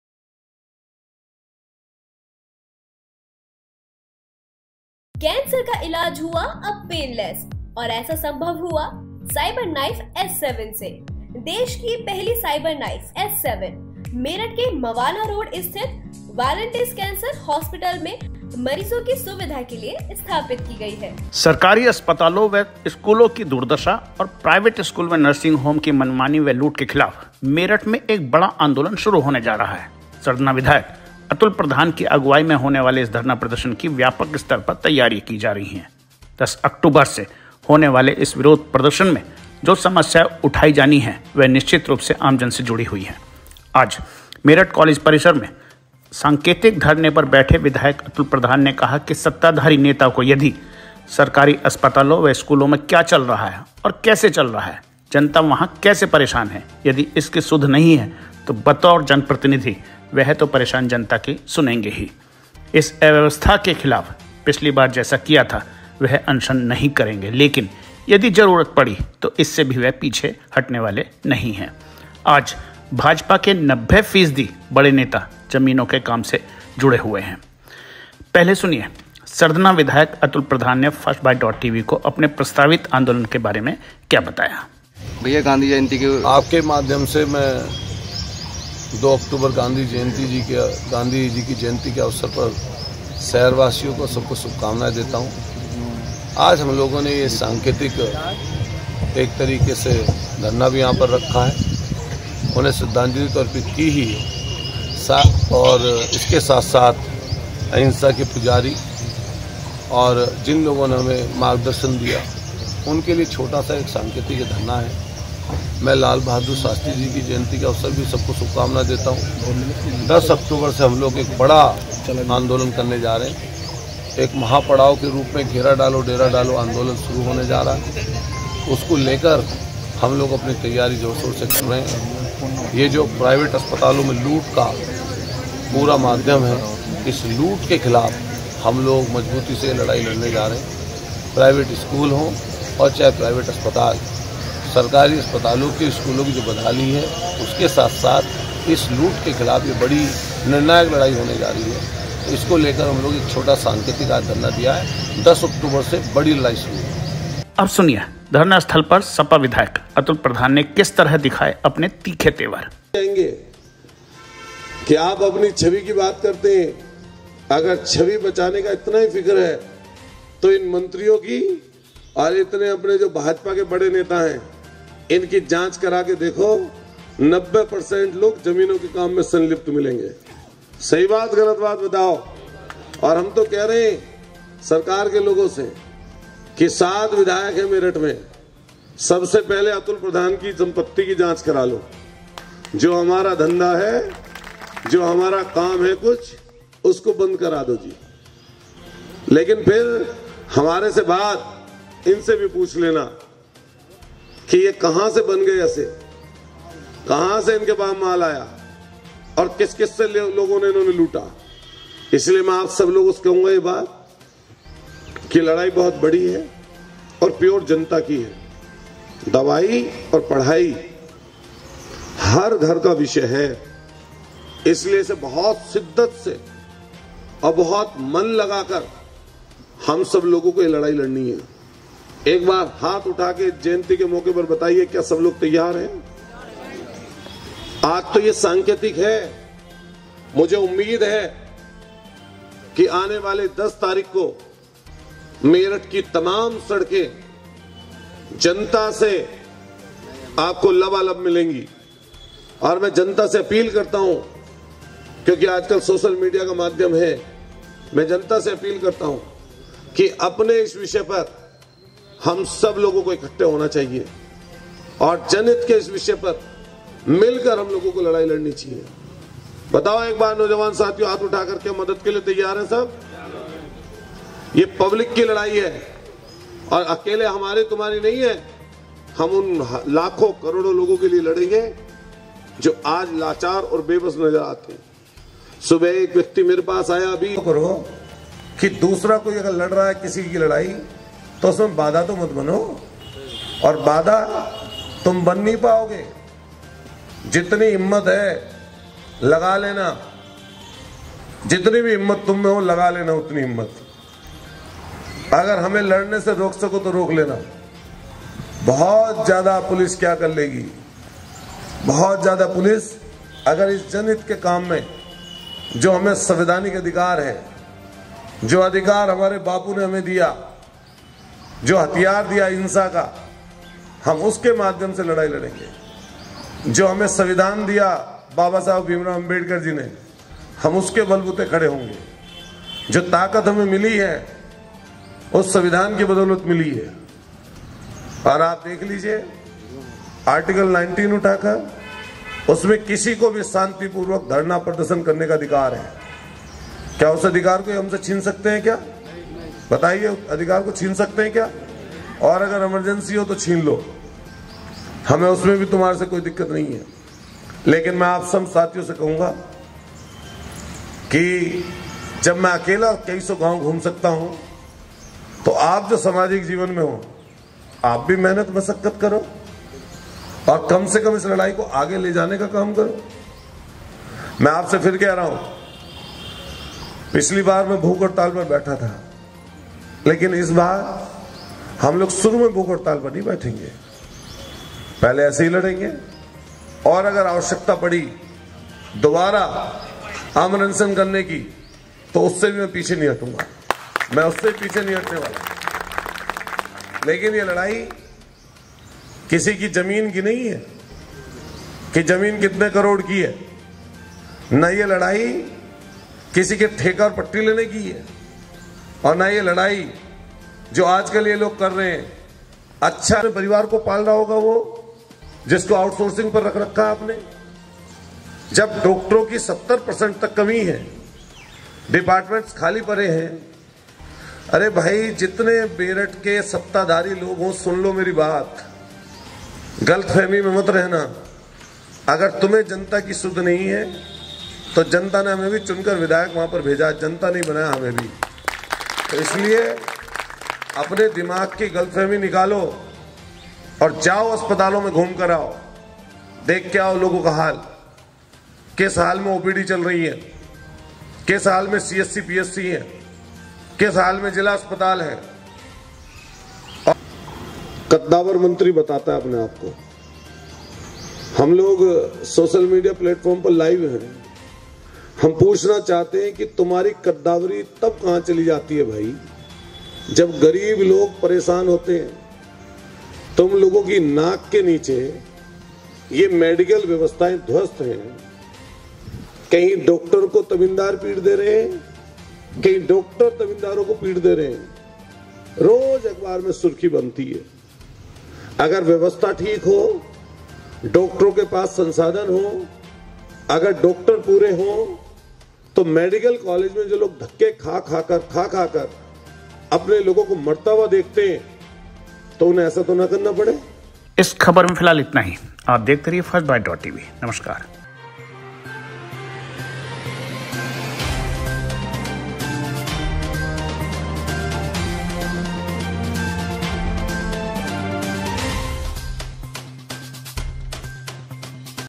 त्रस्त जनता की सत्ताधारी सुध नहीं लेंगे, तो हम तो लेंगे atulpradhan First Byte.tv
प्राइवेट अस्पतालों व स्कूलों के खिलाफ 10 से बड़ा आंदोलन,
सपा विधायक अतुल प्रधान की गांव गांव चल रही तैयारी,
सत्ताधारी नेता परेशान जनता की सुध लेने को तैयार नहीं,
मेडिकल कालेज में रोज हो रही हैं मारपीट की घटनाएं,
भाजपा के 90 फीसदी नेता जमीनों के धंधे से जुड़े हैं-अतुल,
पिछली बार की तरह नहीं करेंगे अनशन, चलेगा धरना,

1.वाकई मोदीजी की तुलना नेहरूजी से नहीं की जा सकती